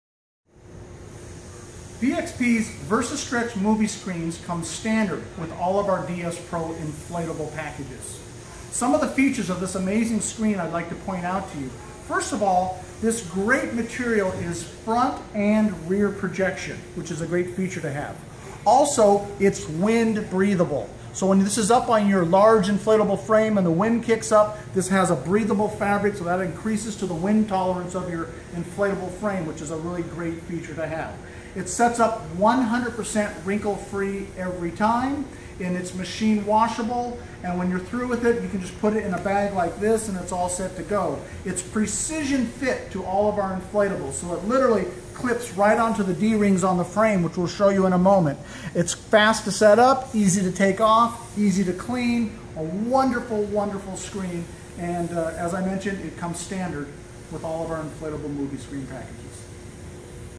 VXP VersaStretch Movie Screen Overview
The features of VXP's VersaStretch Movie Screens. Portable, 100% wrinkle free, front and rear projection and Seamless up the 20ft.